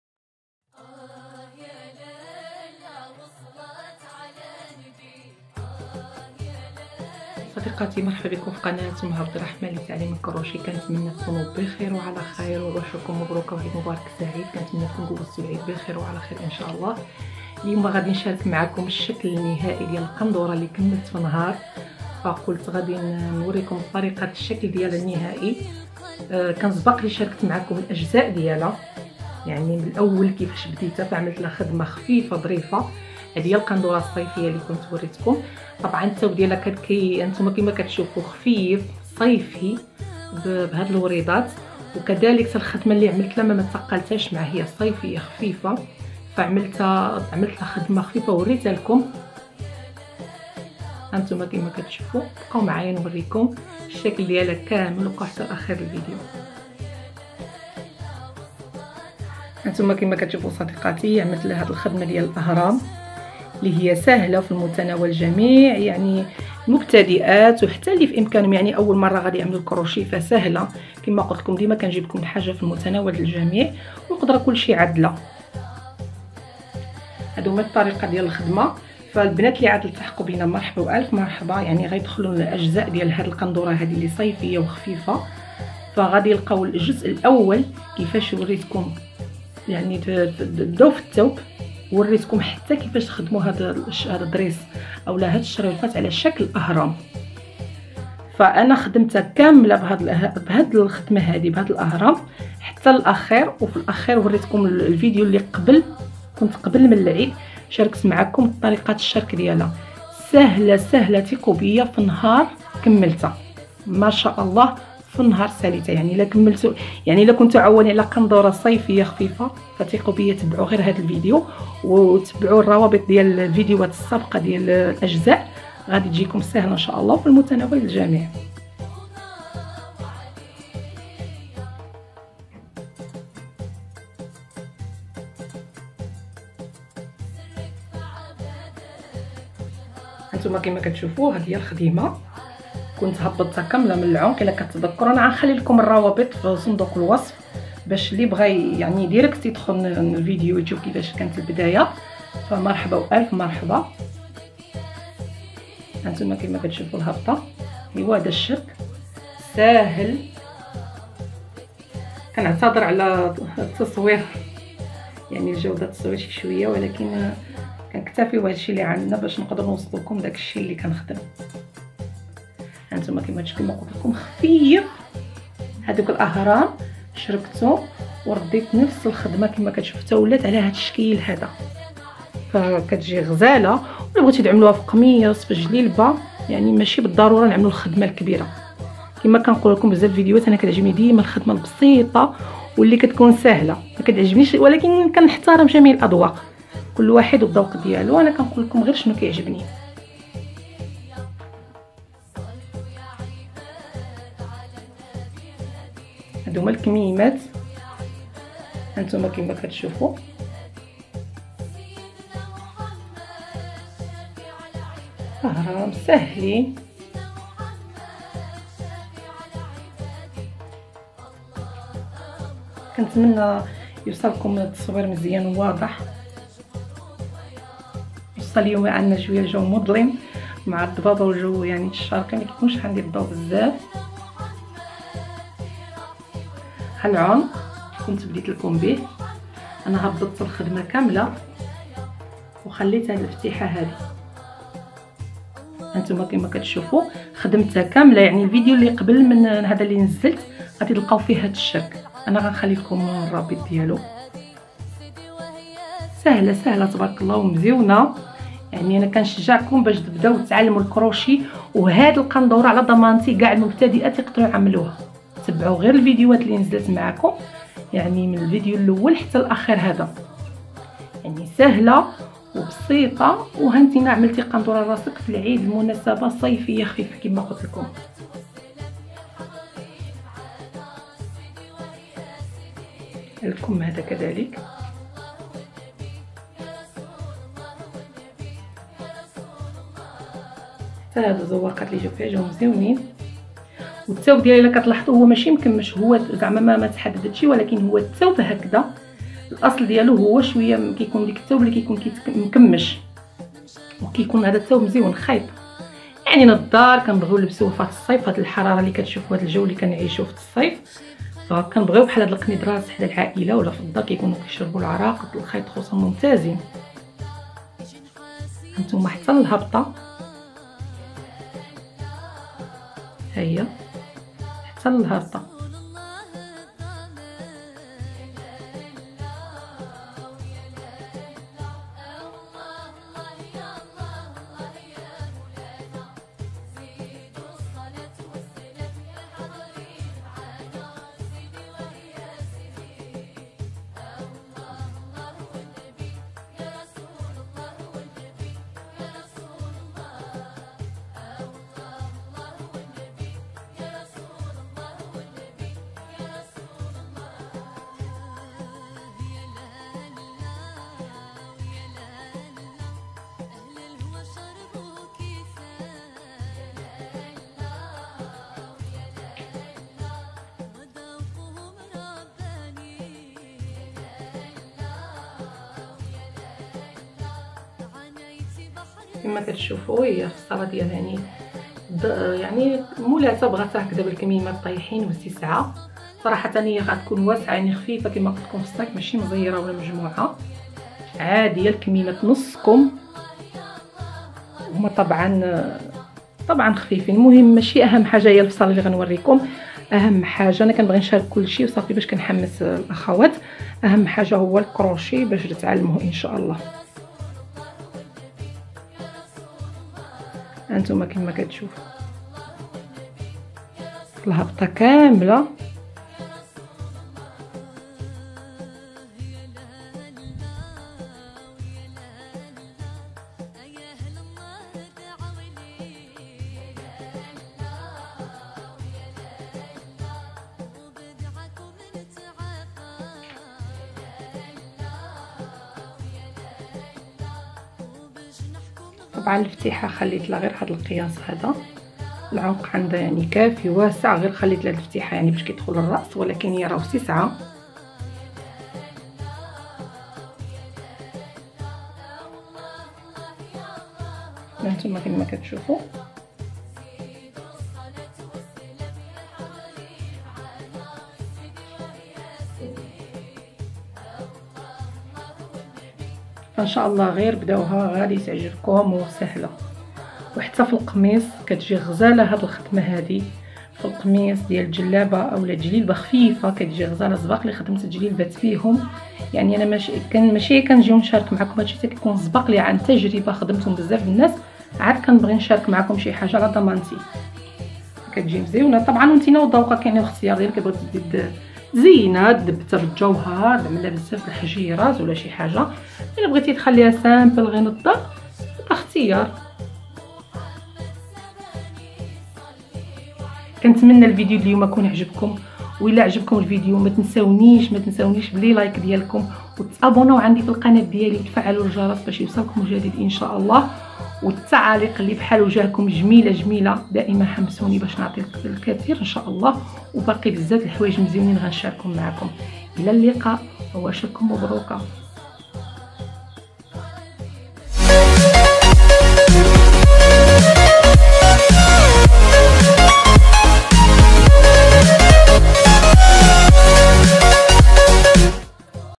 في يوم واحد خدمي قندورة الدار بالكروشي خفيفة وانيقة.crochet 2020
سلام عليكم  اليوم شاركت معكم تتمة قندورة بالكروشي خفيفة وسهلة كنتمن تعجبكم
اسرع  ملاقي بالكروشي